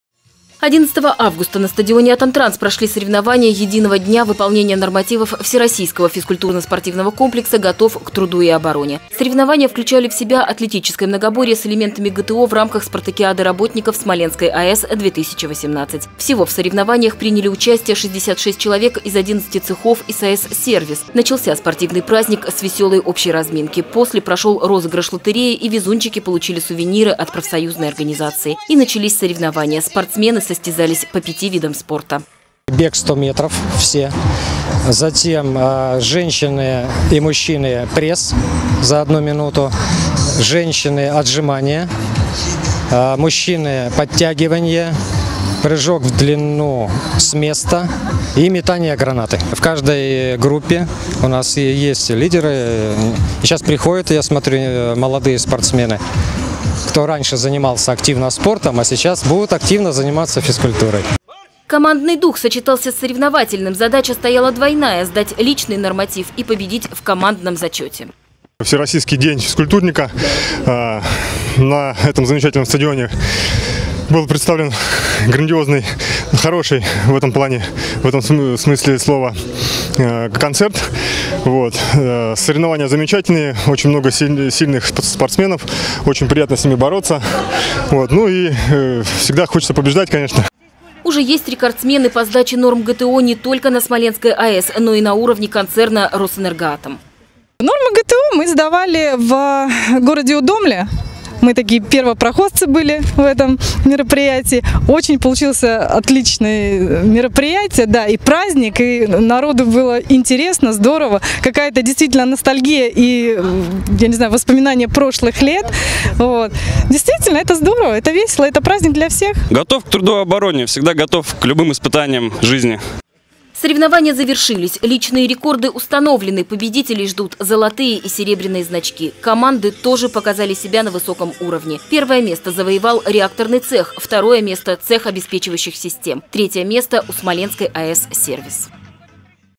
Десна-ТВ: Занимайся физкультурой, если хочешь быть здоров
Спартакиады на Смоленской АЭС проходят дважды в год. Спортсмены соревнуются по зимним и летним видам спорта. С недавнего времени спартакиады включают в себя элементы многоборья ГТО. Это делает соревнования особенно массовыми. Как отметили атомщики Всероссийский День физкультурника, смотрите в сюжете.